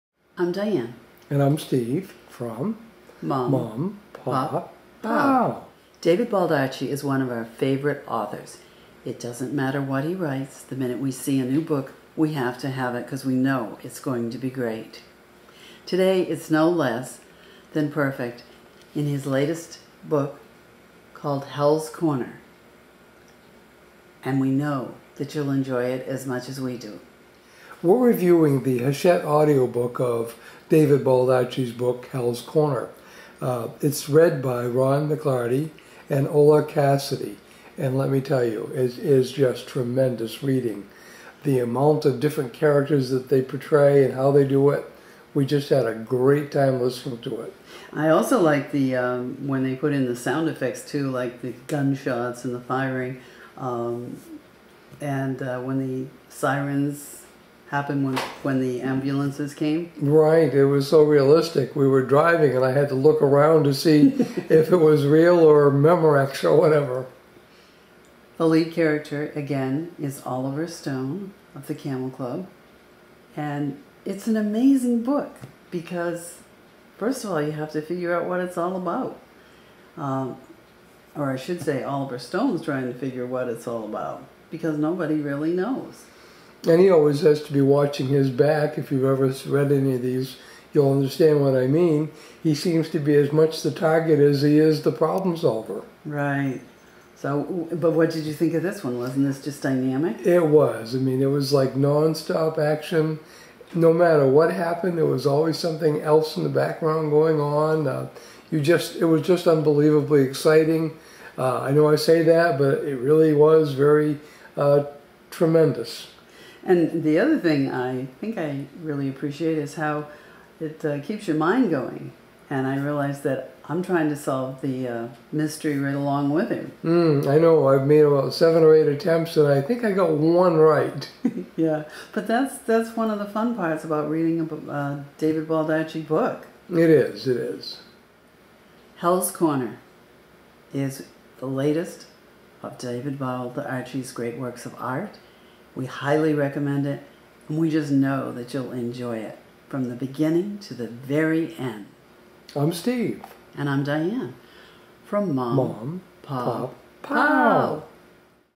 Hells Corner - A Book Review
Hells Corner by David Baldacci is suspenseful, intriguing, surprising, and almost unbelievable.